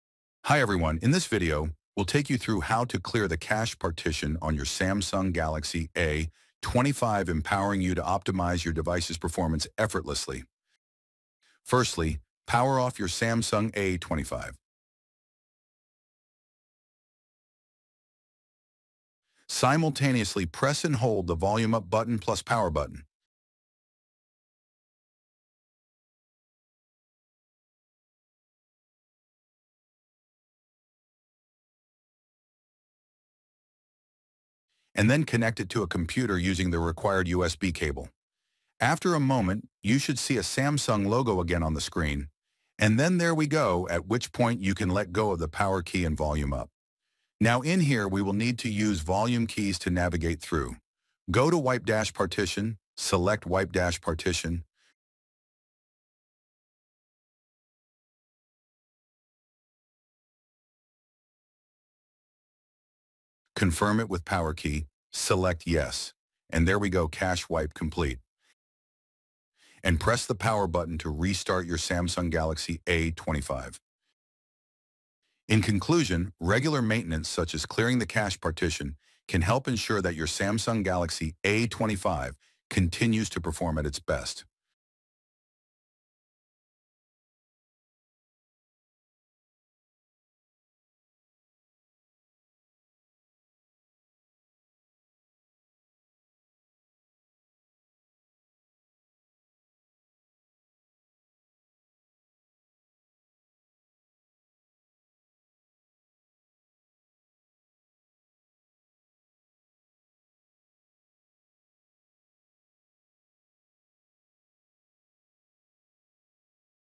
How To Clear Cache Partition on Samsung Galaxy A25
In this video, we’ll take you through how to clear the cache partition on your Samsung Galaxy A25, empowering you to optimize your device’s performance effortlessly.

Utilizing cutting-edge technology, the Samsung Galaxy A25 is robust and feature-rich smartphones. The system cache may build up as you use your device over time, which could cause performance problems.

One helpful maintenance procedure that can help fix a variety of issues and improve the performance of the device is clearing the cache partition. You can learn how to empty the cache partition on the Samsung Galaxy A25 by following this instruction.

How To Clear Cache Partition
Access Recovery Mode
Select Wipe Cache Partition
Confirm the process
Select Reboot System Now